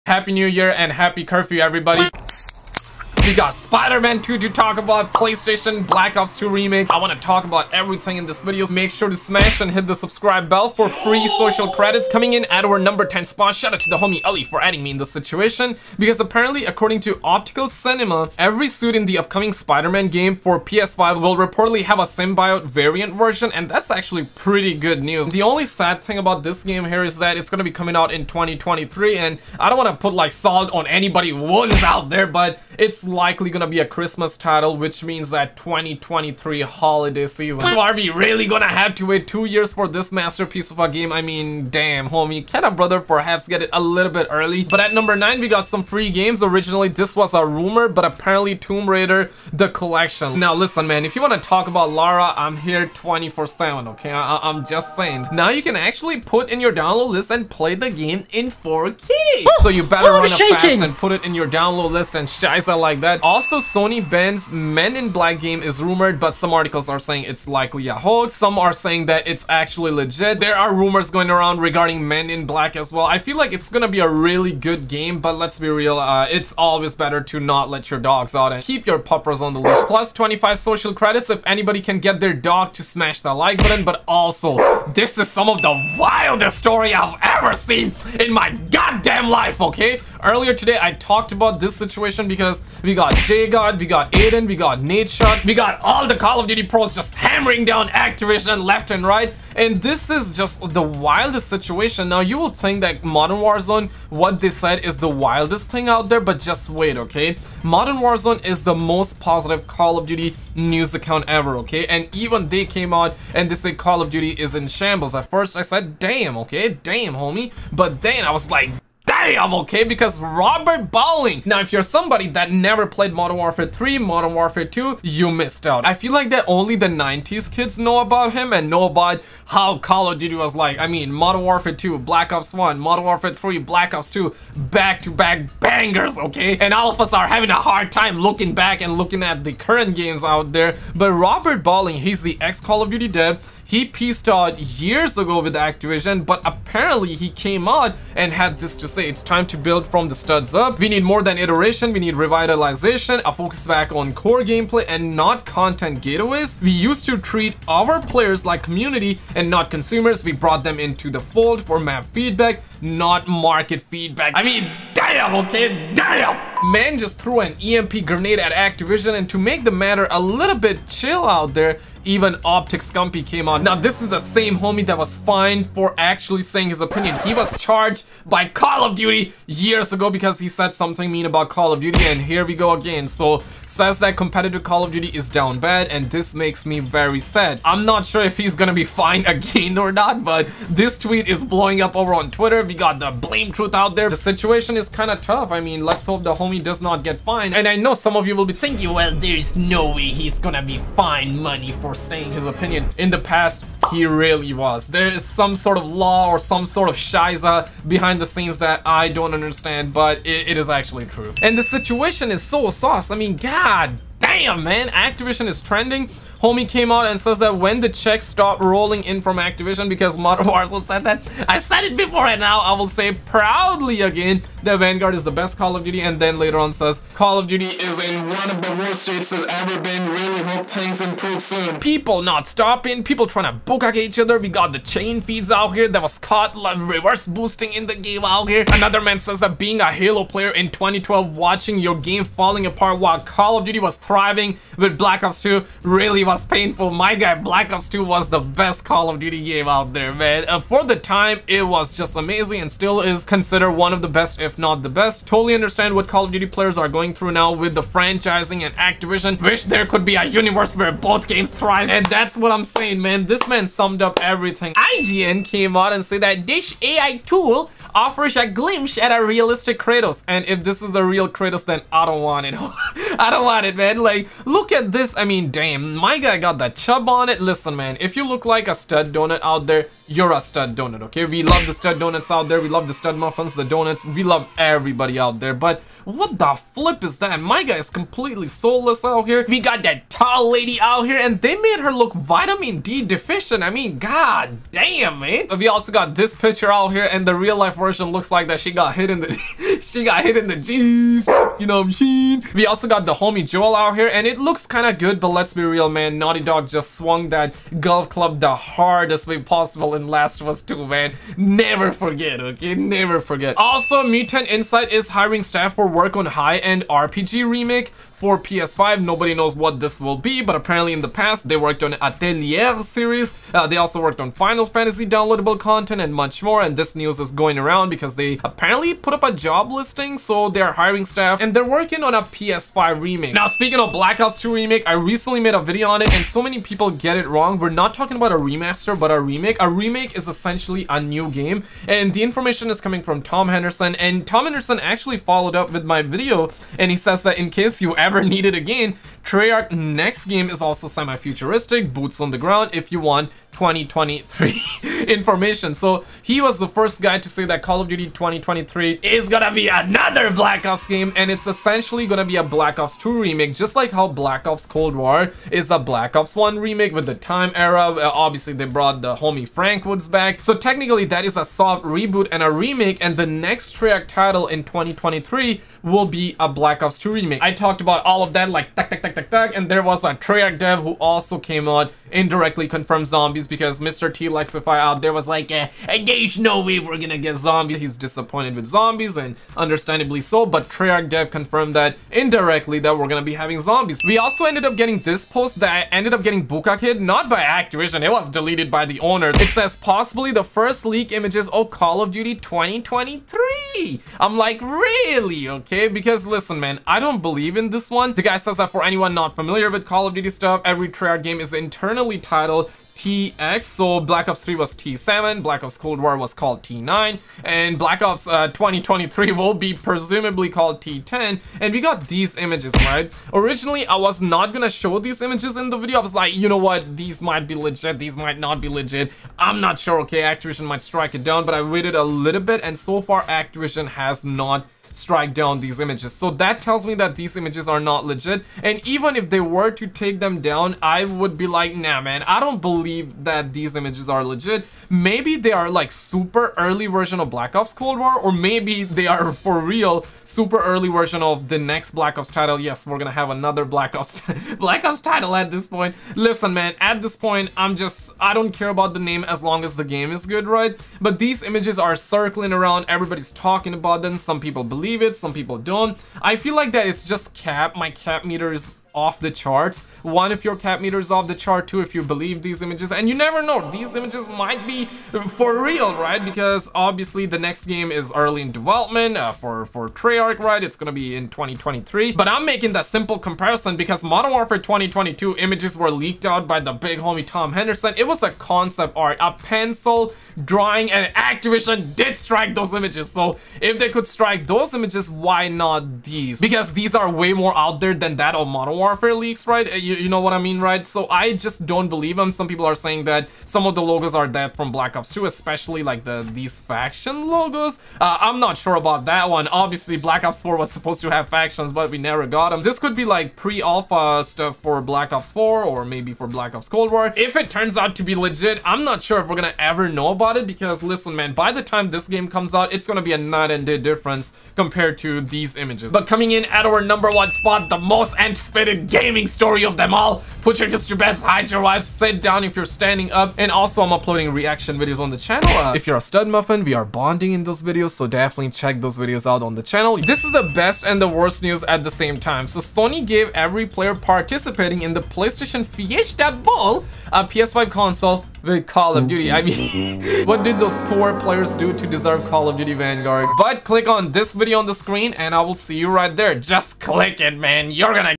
Good News Just DROPPED.. Black Ops 2 😵 - FREE Games, PS5 Spiderman, PS5 Remake
Black Ops 2 REMAKE LEAK, PS5 Spiderman 2 - COD Dev Goes at Activision, Free Games, PS5 Remake.

Today we have a lot of gaming news. We have leaks for Spiderman 2 PlayStation game. We have images for COD 2023 allegedly that I think are not true. We also have Black Ops 2 remake to talk about not Black Ops 2 remastered. Activision is in trouble as Call of Duty players are mad. PlayStation is rumored to be getting Men in Black exclusive game. Mutan is apparently working on a PS5 remake. We also have AI real life version of God of War Kratos, Last of Us Joel, the tall lady from Resident Evil. And Sony gives everyone participating Fiesta Bowl a PS5 console and Call of Duty.